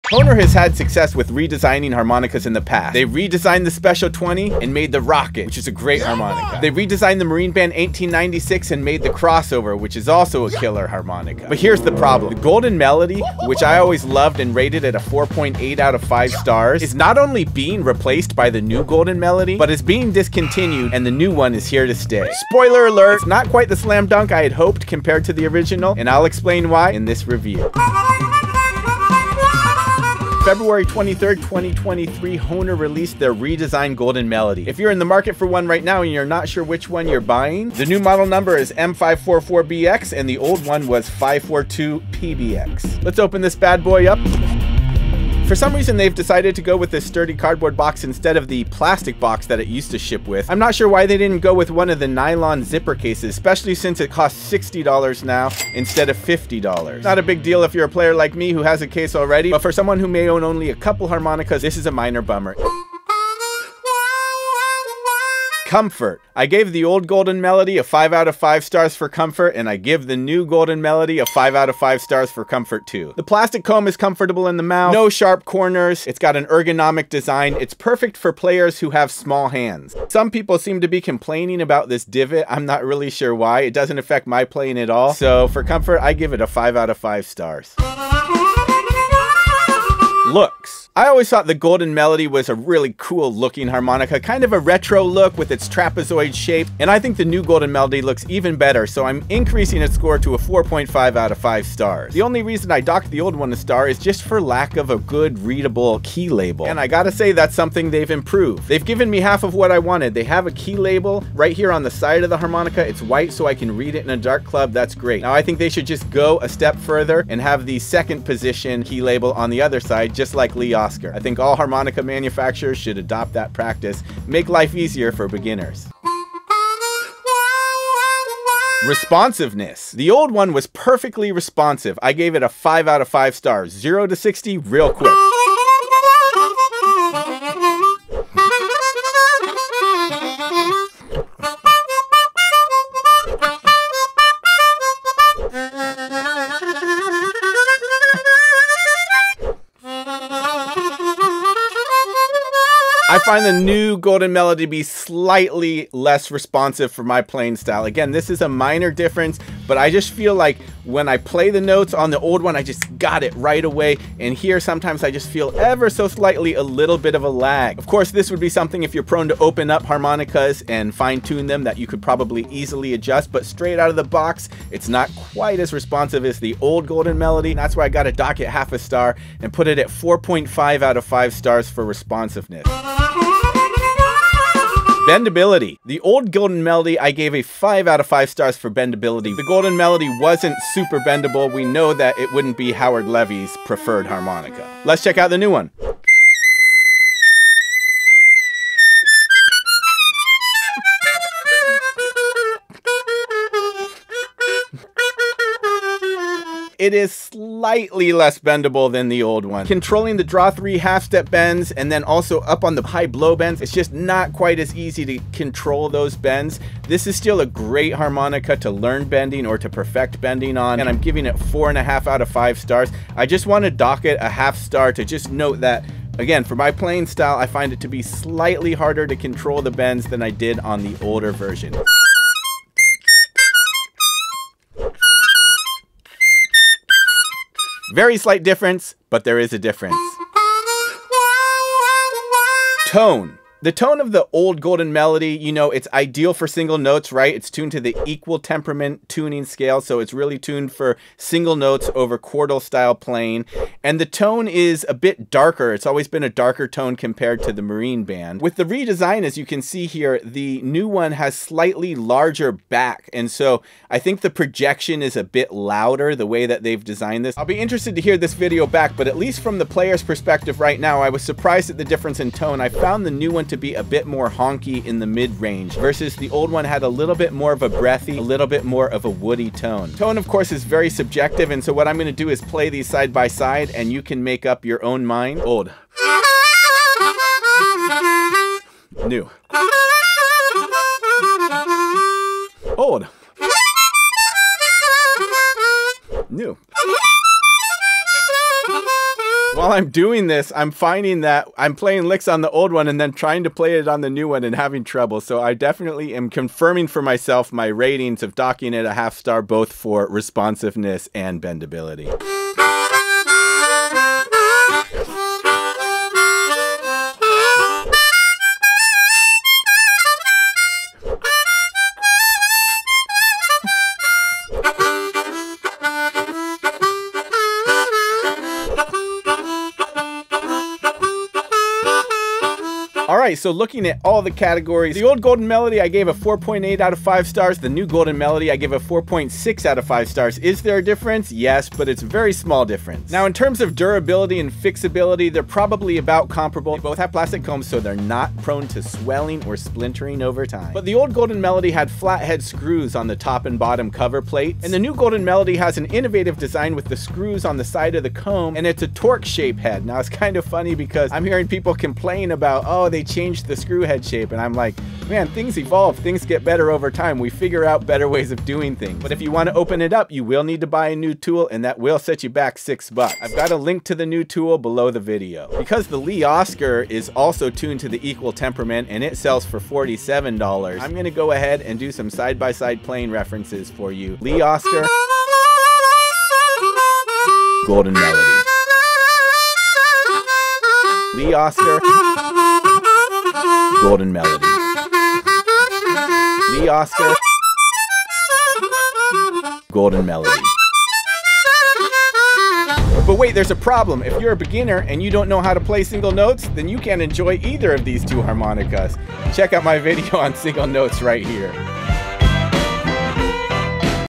The Brand Spankin' NEW Hohner Golden Melody
Here's the tool you need to open her up

On Feb 23rd Hohner released a new redesigned version of their Golden Melody, model # M544BX (the old one was model # 542PBX.)

Why did they redesign it, you ask? Here’s the low down:

The tooling for the old design had aged and needed constant maintenance to continue to produce harps consistently to Hohner’s quality standards. Hohner either needed to build new machinery to manufacture the old design or redesign the Golden Melody.

They decided to take feedback from engineers and players over the years and see if they could make it EVEN BETTER.

NOW to their credit, Hohner HAS had success with redesigning:

The redesigned Special 20 birthed the Rocket
The redesigned Marine Band birthed the Crossover
But the Golden Melody serves a smaller niche market, and the smaller demand wouldn’t warrant having two models of the same harp. So here we are.

With change often comes skepticism,
but then I’m a contrarian, so
I’m skeptical of
the skeptics.

I was fully prepared to love the new design. So…what’s the final verdict???

Well, it’s….ALMOST as good…

For some reason they ditched the plastic box of old and are using a cardboard box? Why not the nice nylon zipper case of the Rockets and Crossovers?
Especially since it now costs $60 instead of $50!
Here’s my ratings of old vs new:

COMFORT
OLD - One of the most comfortable harmonicas on the lips and hands. 5/5 Stars
NEW - Same. I’ve seen people complain about the divet in the cover plate. I”m not sure why. It doesn’t bother me at all. I still love it! 5/5 Stars

LOOKS
OLD - Docked a star for key labels. 4/5  Stars
NEW - I think it looks awesome! Gotta raise it a half star for looks 4.5/5 Stars

RESPONSIVENESS
OLD - Perfect. 5/5 Stars
NEW - Almost as good. Just a little stiff to my tastes. It’s the same for me like the Seydel 1847 Silvers and Suki Manji’s, though not quite as pronounced. It just lags every so slightly for my playing style preference. 4.5/5 Stars

BENDABILITY
OLD - Bends like a champ! 5/5 Stars
NEW - Almost as good. Not QUITE as easy as the older model or SP20. Very minor difference.  4.5/5 Stars

TONE
OLD - The tone of this harmonica is dark and beautiful. Ideal for single notes.. 5/5 Stars
NEW - Almost as good. It’s got more of a midrange honk. 4.5/5 Stars

TOTAL
OLD - 4.8/5 Stars
NEW - 4.6/5 Stars

Durability/Fixability – The plastic comb is not susceptible to any of the swelling or splintering that a wood comb could do over time.  The old model had 2 flat head screws per cover plate, and the new model has hex screws on the side.

It’s funny to me how up-in-arms some are about the switch to a Torx screw.
Come on guys, it’s a $6 tool and it’s a better head style than a flathead! (I’m pretty sure that’s called progress? 🤔)

We all have different preferences. The great Rachelle Plas LOVES the new design and says she can get a much better dynamic range from the new model playing soft nuanced parts as well as louder projected parts. She’s one of the best, so her vote matters! Different strokes for different folks…
The newly designed Golden Melody is still a great harmonica at 4.6/5 stars. It’s still my top Hohner recommendation for pure single note players, and especially for those with small hands.

HOWEVER, because the Lee Oskar is also tuned for single notes, and is only $47 versus the $60 for the Golden Melody, I vote Lee Oskars the best value pro harmonica for single note playing.

Aloha,
Luke